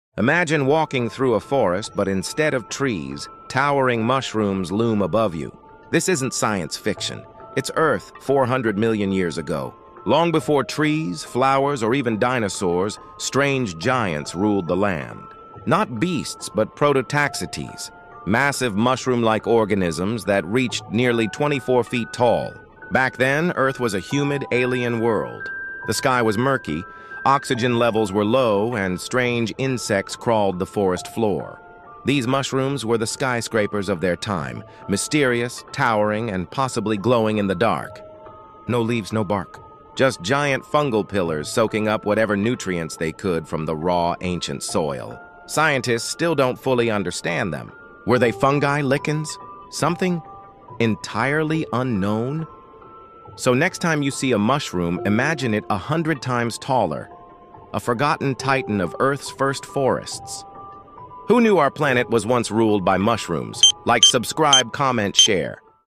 Gaint Mushrooms#glowinthedark#earthtimeline#evolution#earlylifeonland#fungi#biologyshorts#usa#india
Before trees… before dinosaurs… Earth was covered in towering 24-foot mushrooms called Prototaxites! 🌍🍄
These ancient fungal giants ruled the land 400 million years ago — no trees, no flowers, just mushroom skyscrapers in a mysterious, alien world.
Scientists still debate what they really were… fungi, lichens, or something totally unknown? 🤯
👉 Watch now to explore Earth’s weirdest history — and hit LIKE if you’d survive in a mushroom jungle!